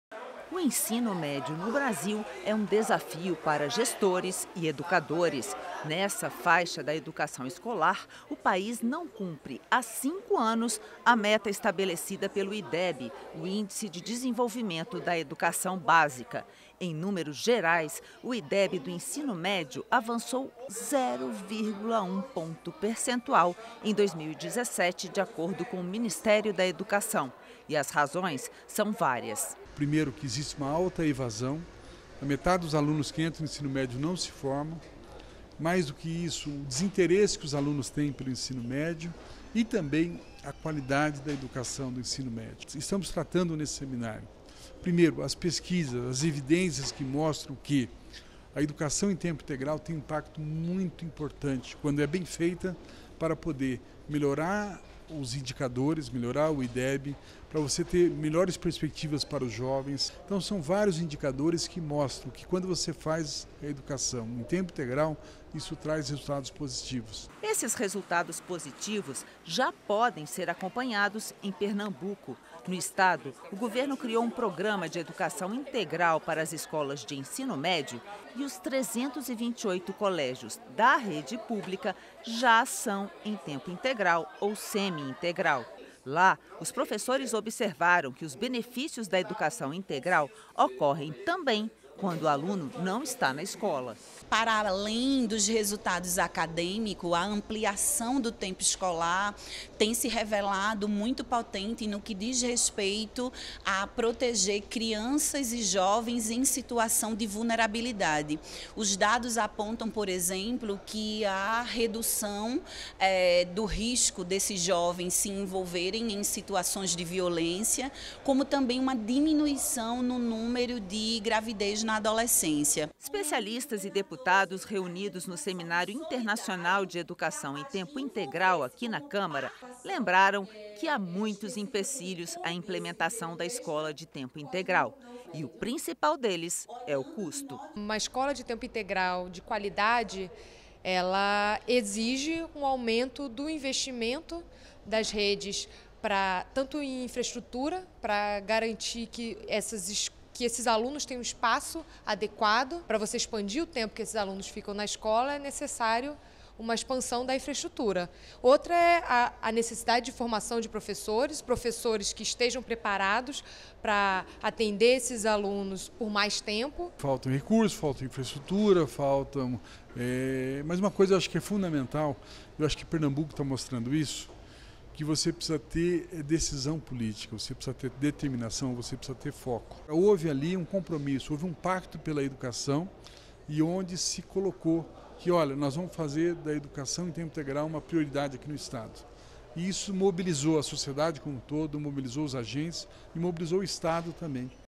Seminário discute ampliação do ensino integral - 12/12/18
Experiências com o ensino em tempo integral que deram certo, no Brasil e no mundo. Os exemplos foram apresentados em seminário nesta quarta aqui na Câmara. O Plano Nacional de Educação determina que metade das escolas públicas ofereçam educação em tempo integral até o ano de 2024.